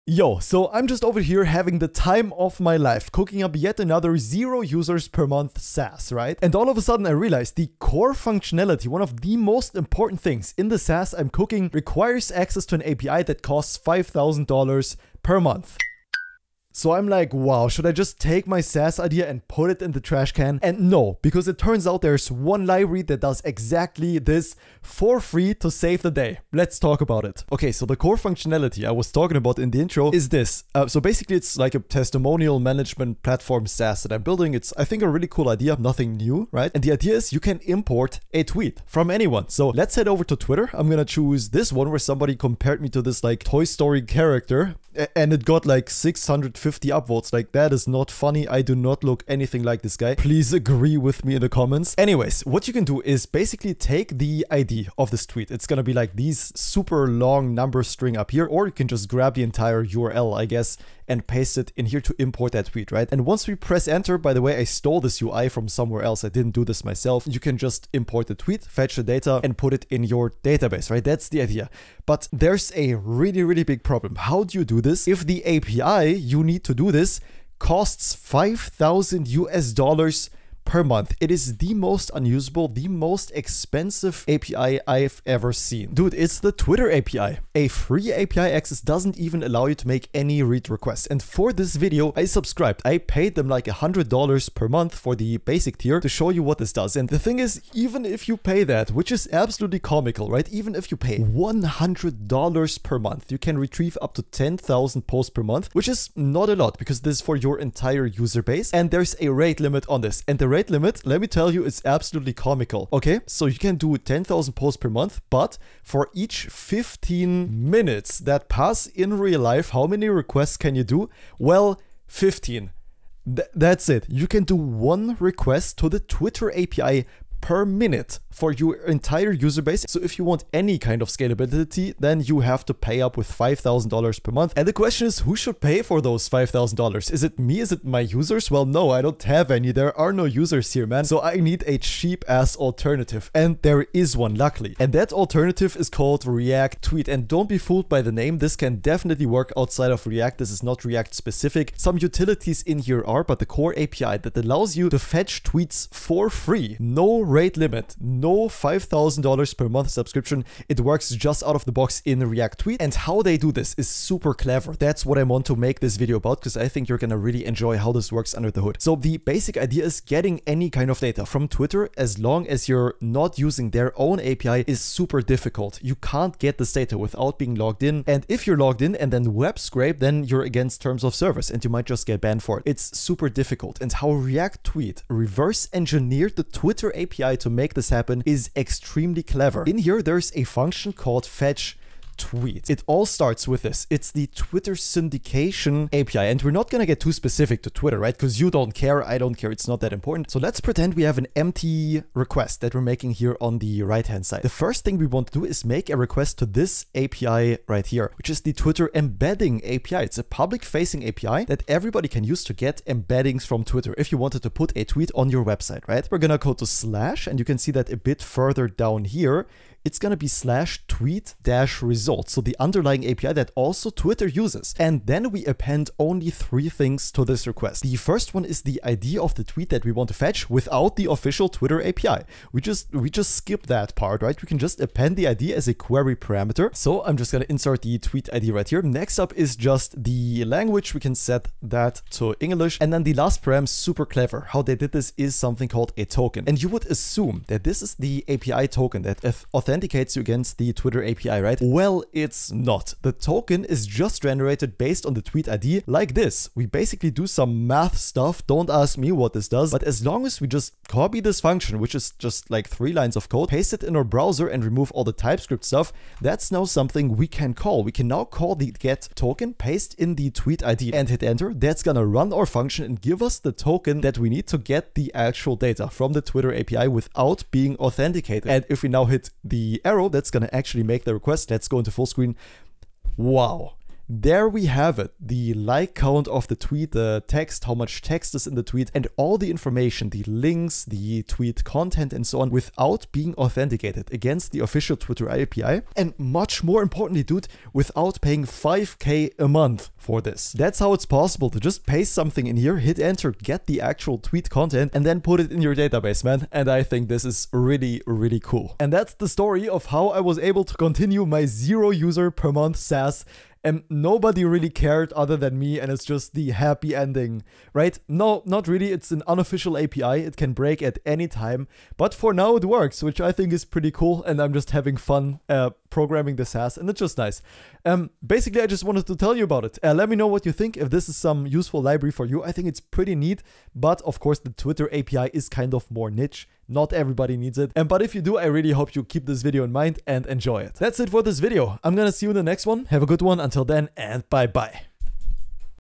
This Package Saved My SaaS
depending on a third-party api for your saas is not fun, because, well, it gives that third party the power to charge pretty much ANYTHING they want. As is the case with twitter. This is a pretty smart solution